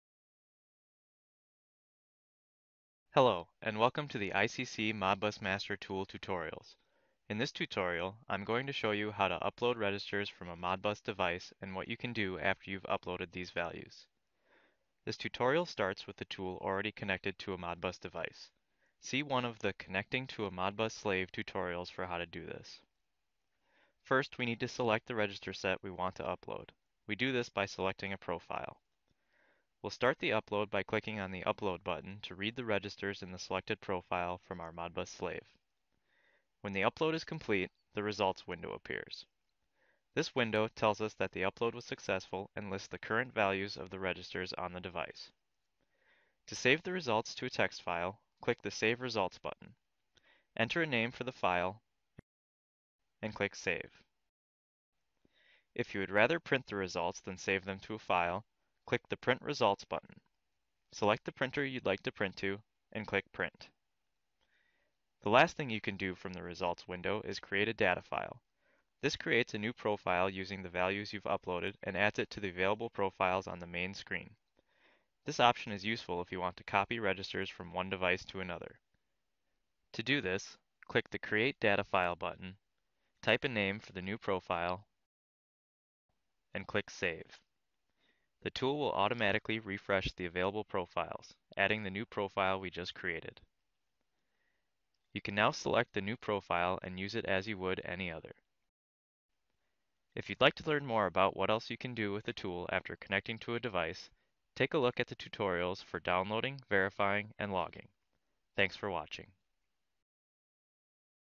Uploading Registers
This tutorial describes how to upload registers from a Modbus device once the tool is connected to it.